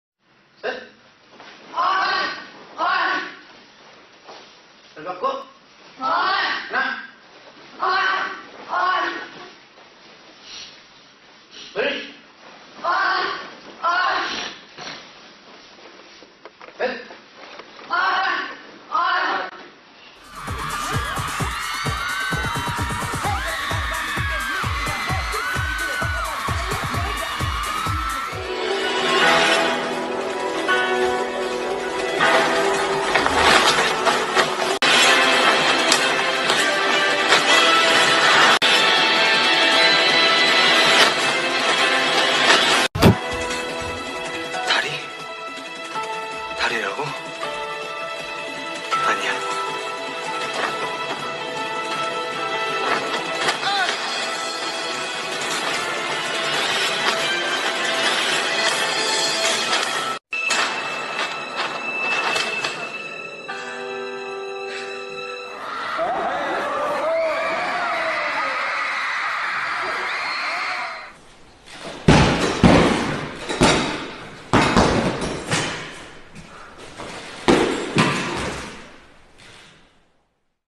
BTS [방탄소년단] Jungkook Taekwondo [정국이 태권도 모음]- Compilation of Jungkook doing Taekwondo
here is a short compilation of BTS Jungkook doing Taekwondo

Credit to BTS and justjeongguk